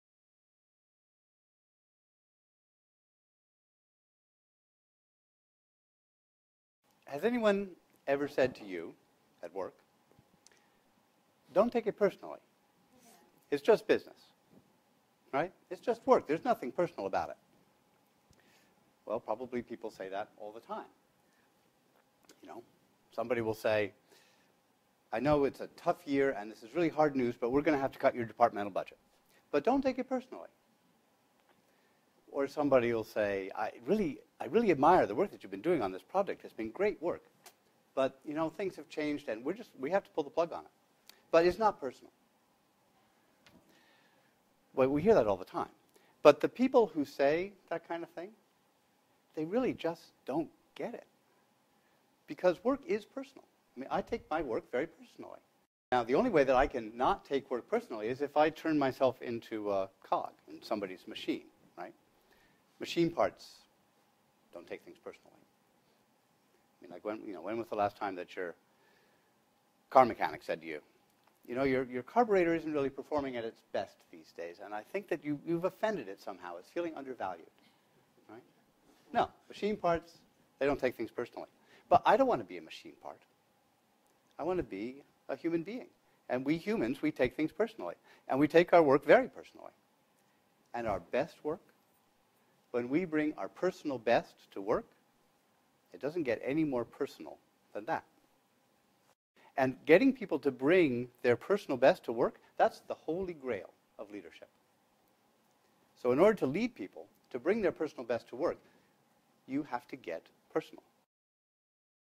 Crane Stookey - Work is Personal-
"Tall Ship officer, Founder of the Nova Scotia Sea School, recipient of the Queen's Jubilee Medal, who brings "crew-building" strategies ashore for your organization." Have Crane speak at your next event.